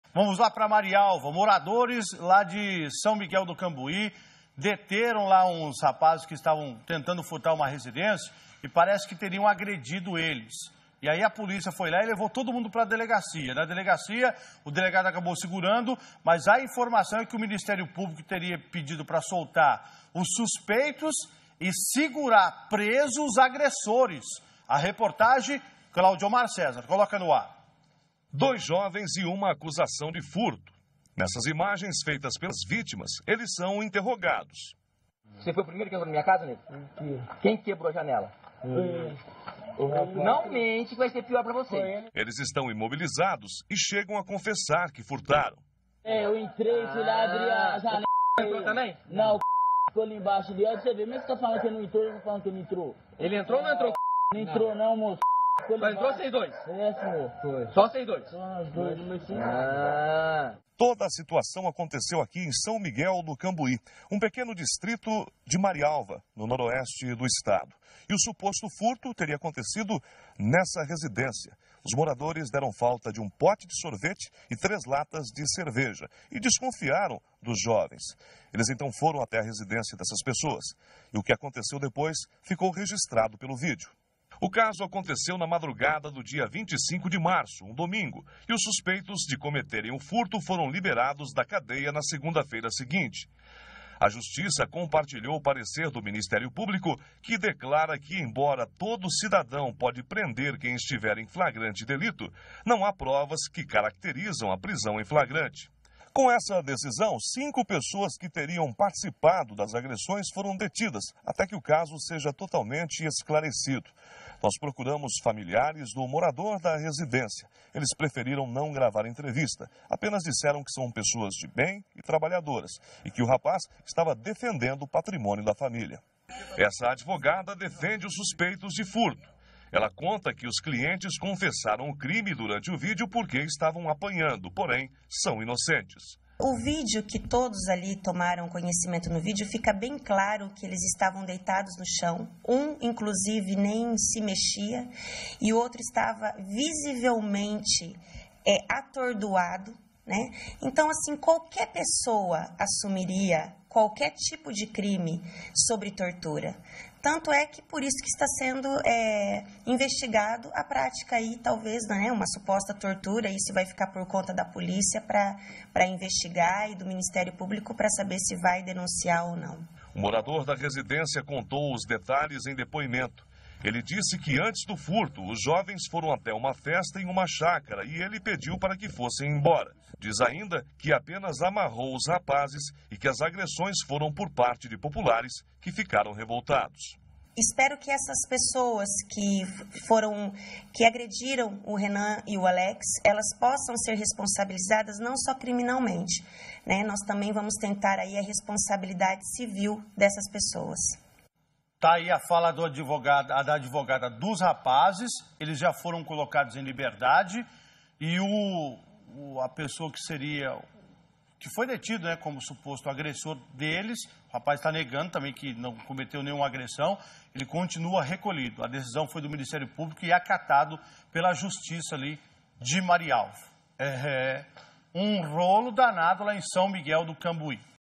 Moradores são presos por agredirem suspeitos de assalto em Marialva
A situação aconteceu em São Miguel do Cambui, um distrito de Marialva. O suposto furto aconteceu em uma residência de onde teria sido um pote de sorvete e três latas de cerveja. Quando perceberam que os produtos haviam sido furtados, os moradores desconfiaram dos dois jovens e foram até a casa deles para tirar satisfação.